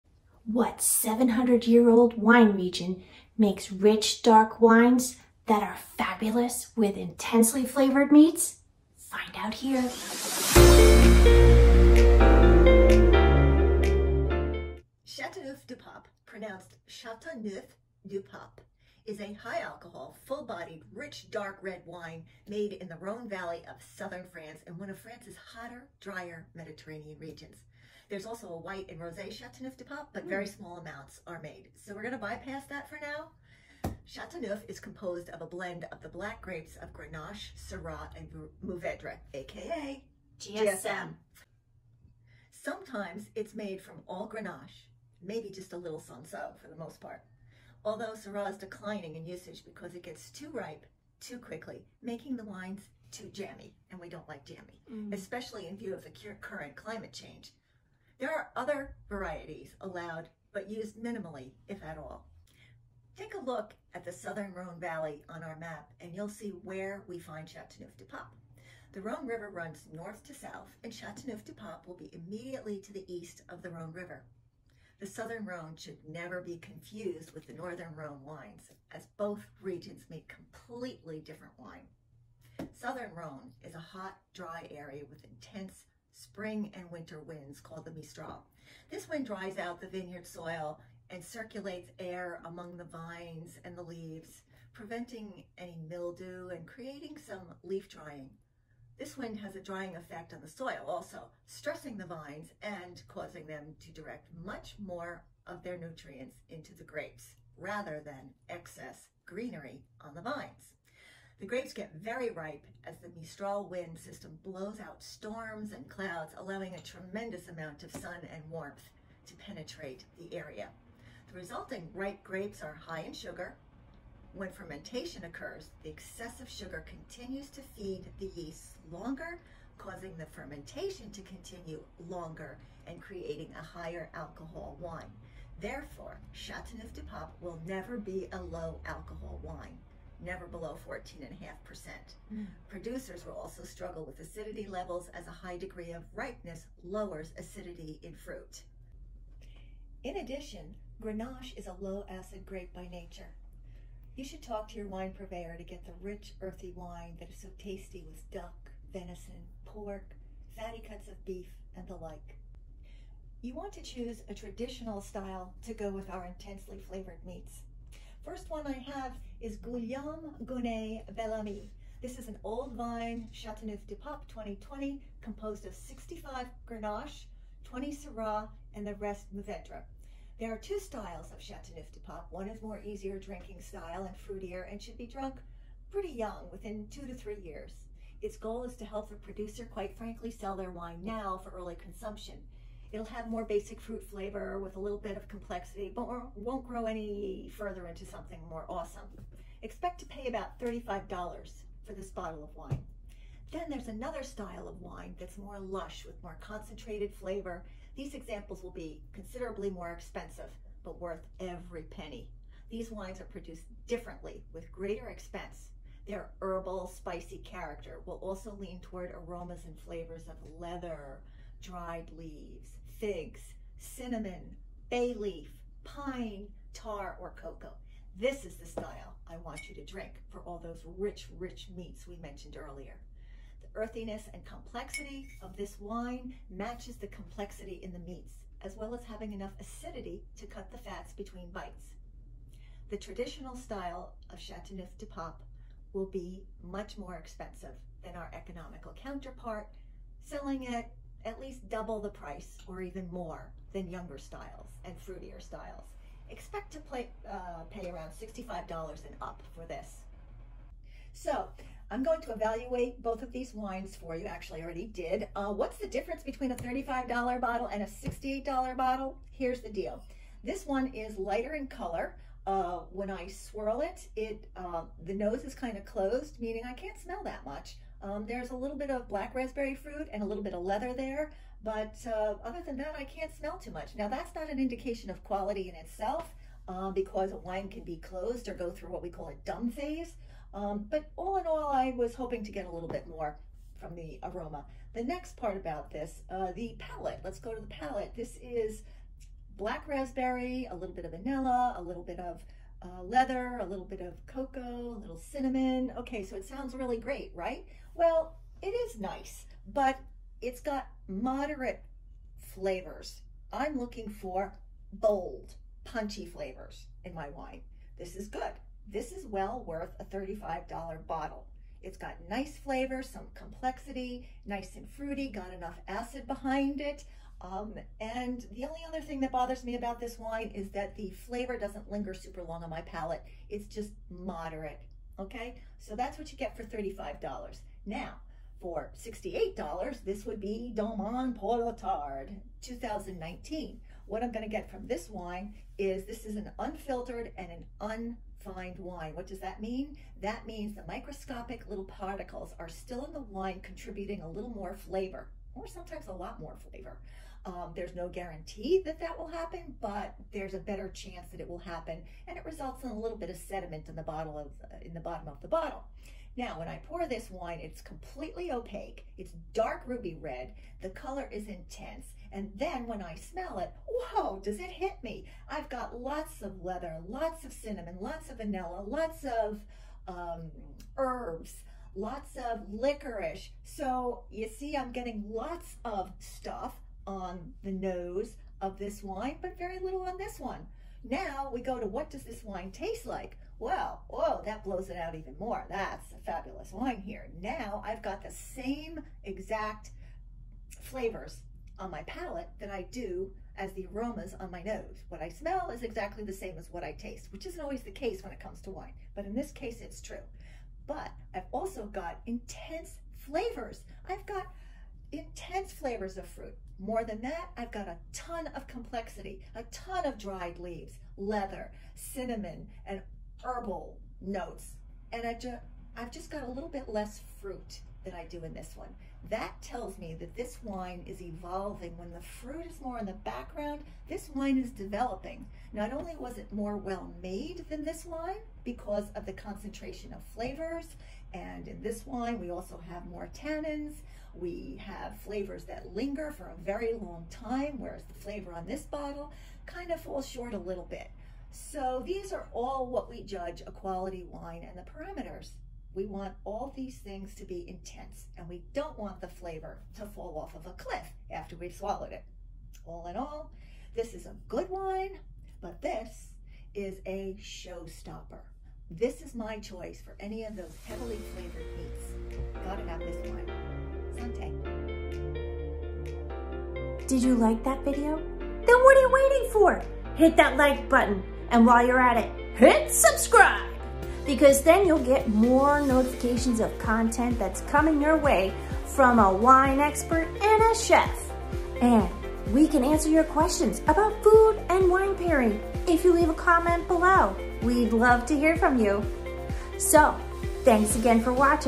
Discover Muscle-Head Red Wines!
Do you know what wine to have with meats like duck or beef? We have the right pairing for you! These two Chateauneuf-Du-Pape red wines are differently delicious! Marta gives you the details on both wines, the 700 year old wine region, and why these work.

Don’t miss a Sip of the Fun!

Marta Marchetti-Kandl, DipWSET
Becky Rapp, AAS, The Culinary Institute of America

Be sure to check us out:

 (Wine Tasting Notes and Recipes too!)

0:00-0:14 Intro
0:15-2:28 Characteristics
2:29-2:42 Grenache
2:43-4:15 Selecting
4:16-7:54 Wine Tasting
7:57-8:41 Outro